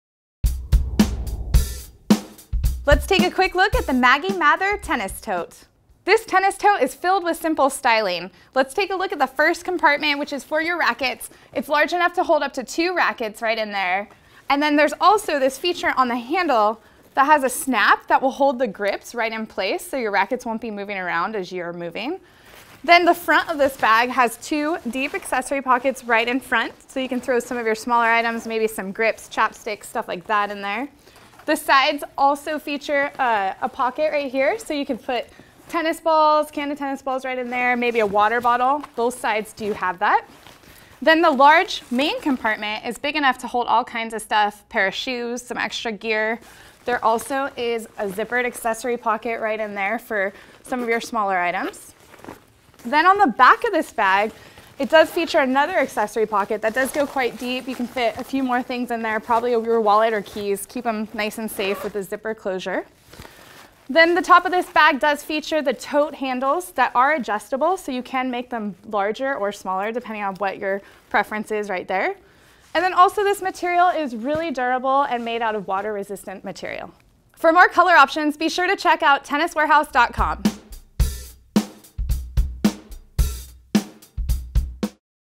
Maggie Mather Tennis Tote Bag
Arrive to the courts in style with this sturdy and stylish tennis Maggie Mather tennis tote. The high quality canvas bag features water repellent fabric and a leather Maggie Mather logo. Easily carry two racquets in a separate compartment and secure them through the throat with a strap. You can fit plenty of items in the smaller outer pockets, and a large main compartment ensures you won't run out of room. Throw this one over your shoulder and adjust the straps to fit you comfortably. This Maggie Mather bag is so stylish you can use this one for tennis, workouts or shopping.